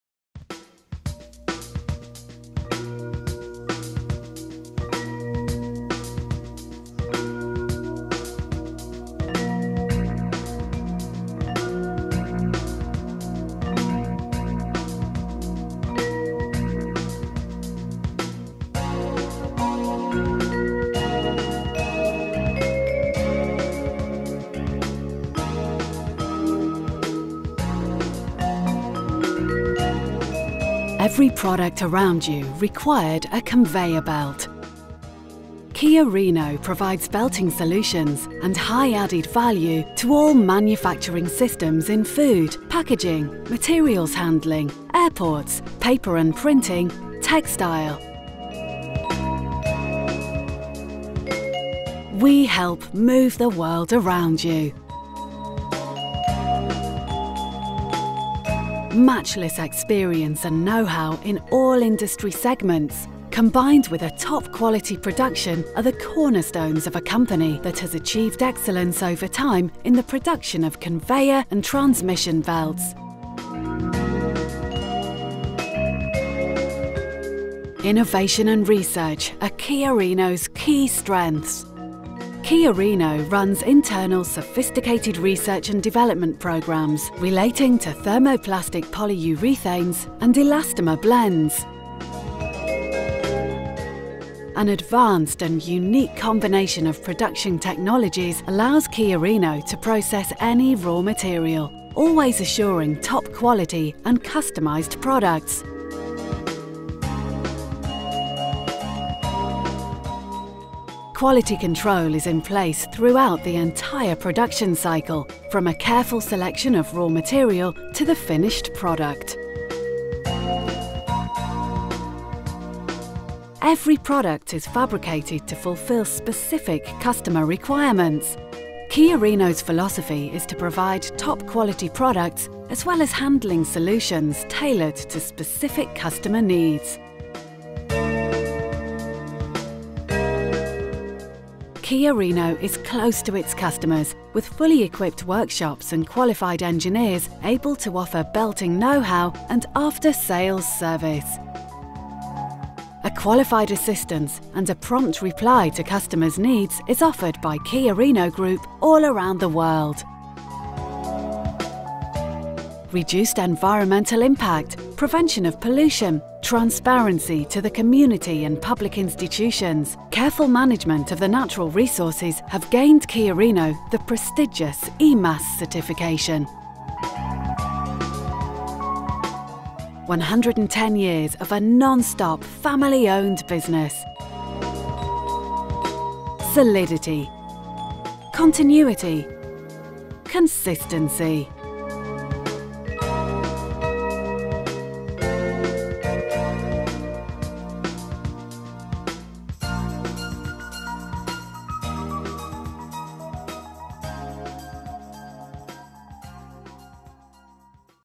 Chiorino Group Corporate
Every product around you required a conveyor belt.
Chiorino provides belting solutions and high added value to all manufacturing systems in food, packaging, materials handling, airports, paper & printing, textile...
We help move the world around you.
Matchless experience and know how in all industry segments, combined with a top quality production are the cornerstones of a company that has achieved excellence over time in the production of conveyor and transmission belts.
An advanced and unique combination of production technologies allows Chiorino to process any raw material, always assuring top quality and customized products.
Innovation & Research are Chiorino’s key strengths.
Chiorino runs internal sophisticated research & development programs relating to thermoplastic polyurethanes and elastomer blends.
Conveyor and transmission belts are designed to guarantee reduced production costs, energy saving and total compliance with the latest stricter regulations.
Chiorino’s philosophy is to provide top quality products, as well as handling solutions tailored to specific customer needs.
Thanks to its many different production technologies, Chiorino can offer application-oriented solutions in any market segment.
Quality control is in place troughout the entire production cycle, from a careful selection of raw material to the finished product.
Every product is fabricated to fulfill specific customer needs.
Chiorino is close to its custormers, with fully equipped workshops and qualified engineers able to offer belting know how and after sales service.
A qualified assistance and a prompt reply to customers needs is offered by Chiorino Group all around the world.
Reduced environmental impact, prevention of pollution, transparency to the community and public institutions, careful management of the natural resources have gained to Chiorino the prestigious EMAS certification.
110 years of a non-stop family owned business.
Solidity. Continuity. Consistency.